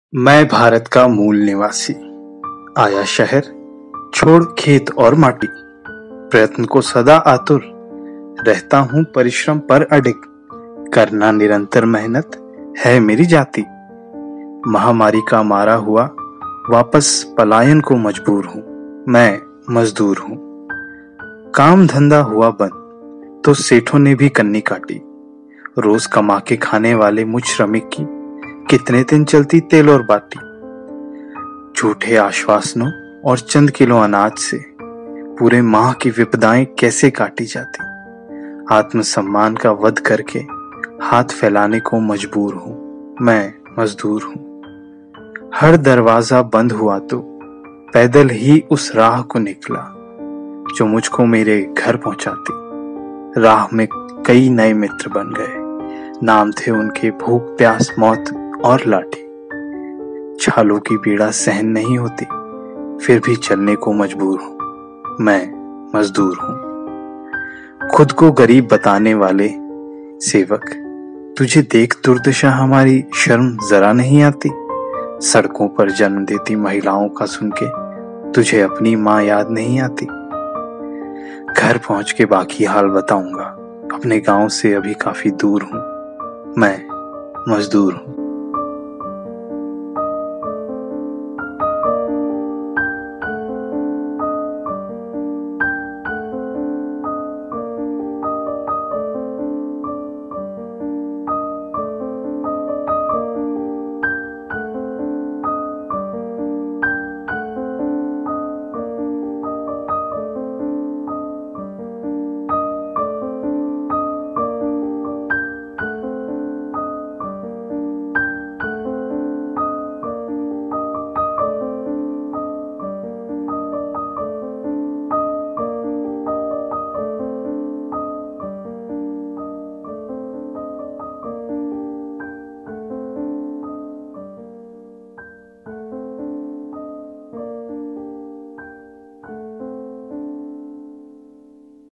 Ep.07 मैं मज़दूर हूँ Ft. रवि नागर | Mohitbuzz | Migrant Workers पर कविता | Poetry | Mai Majdoor Hoon
I know this is not "TRENDING" anymore, and why would people watch something which is not trending, but one of my friends recorded this poem and asked me to put it on my social media platforms!

Hope you like it - hope you join us on the Mohitbuzz to more such hard-hitting videos.

Video Editing: Mohit Gupta